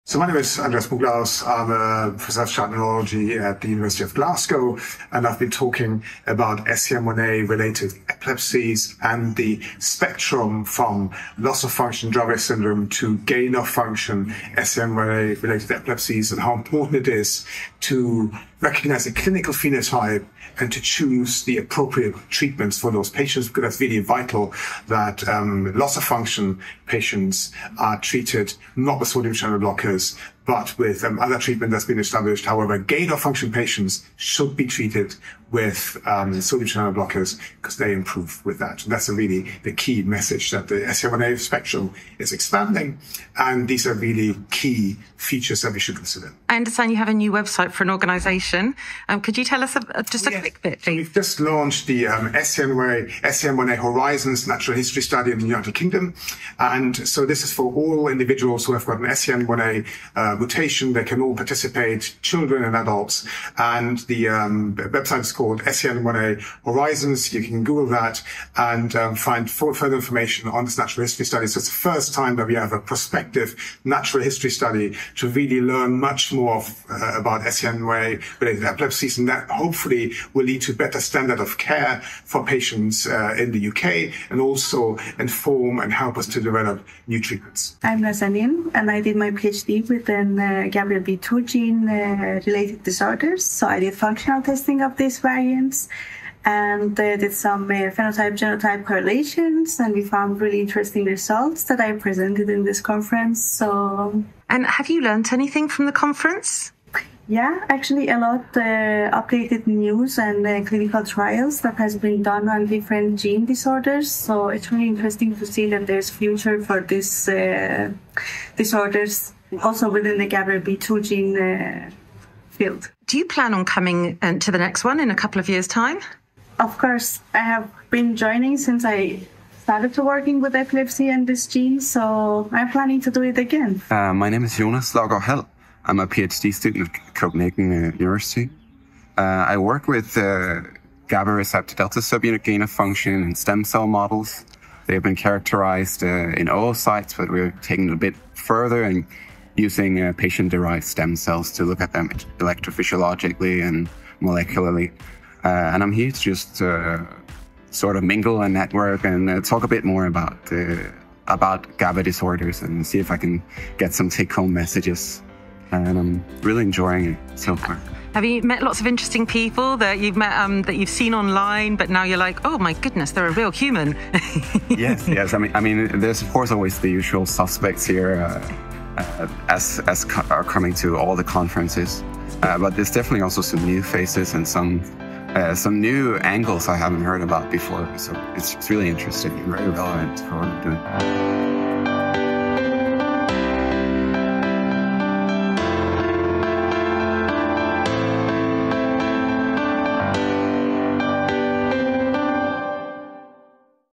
DICE 2024 Reviews: Andreas Brunklaus, Nazanin Azarinejad Mohammadi, & Jonas Laugård Hald
Star reviews of the 6th Dianalund International Conference on Epilepsy, 2024: “Overlapping clinical phenotypes in monogenic epilepsies – common molecular pathways?”.

00:00 Andreas Brunklaus
01:28 Nazanin Azarinejad Mohammadi
02:14 Jonas Laugård Hald
03:11 The 2026 DICE conference

to keep up to date with the latest developments.